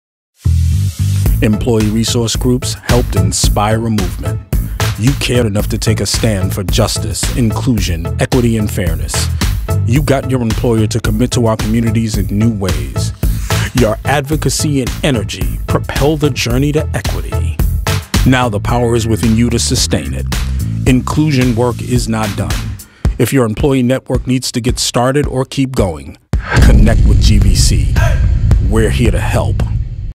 Employee Resource Groups Helped Inspire a Movement
Employee Resource Groups had the courage to do what's right and changed corporate America. Inclusion Work is Not Done. Whether you need to get started or keep going, GVC is here to help.

Connect with GVC at diversitystrategy.com.